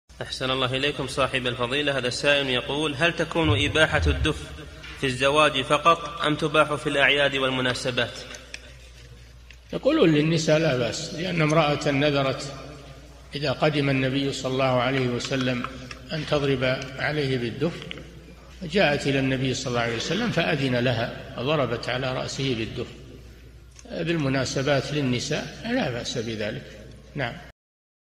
[96 -954] هل تكون إباحة الدُّف في الزواج فقط، أم تُباح في الأعياد والمناسبات؟ الشيخ صالح الفوزان
-[96 /954] السؤال: هل تكون إباحة الدُّف في الزواج فقط، أم تُباح في الأعياد والمناسبات؟
الجواب: يقولون: للنساء لا بأس، لأن امرأةً نذرت إذا قَدِمَ النبي ﷺ أن تضرب عليه بالدُّف، فجاءت إلى النبي ﷺ فأذن لها فضربت على رأسه بالدُّف بالمناسبات للنساء فلا بأس بذلك.
للاشتراك في قناة الشيخ صالح الفوزان
سؤال وجواب من شرح منظومة الآداب ( 945 ) مقطع

للاستفسار والإقتراحات والدعم
رئيس الفريق
لمشروع كبار العلماء